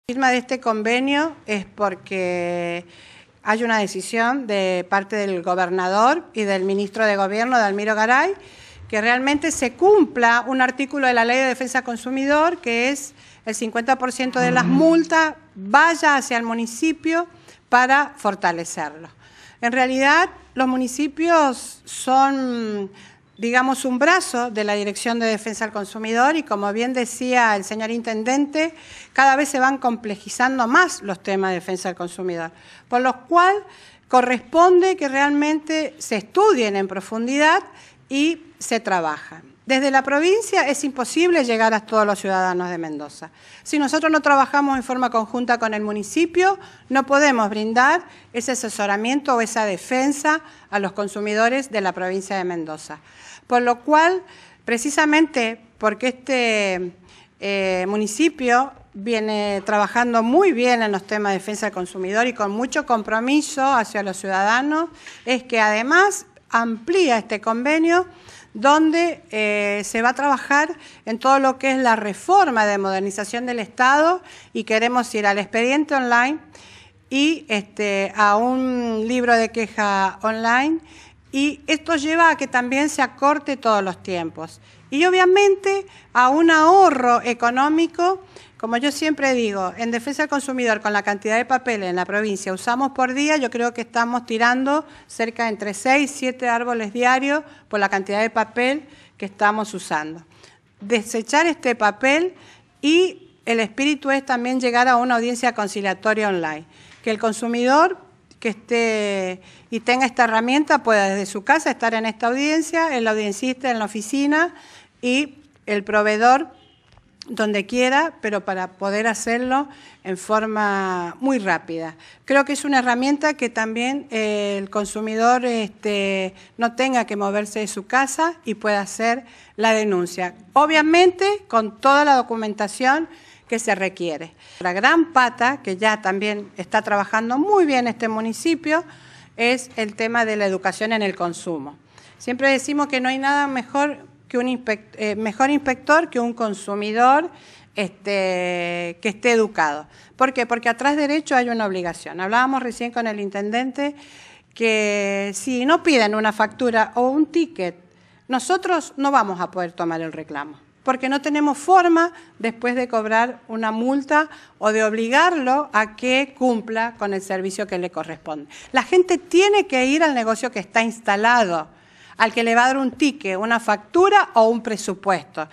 UNIÓN PROVINCIA Y MUNICIPIO EN DEFENSA DEL CONSUMIDOR
La Directora Provincial de Defensa del Consumidor Mónica Lucero de Nofal informa sobre el convenio renovado con la Municipalidad de San Rafael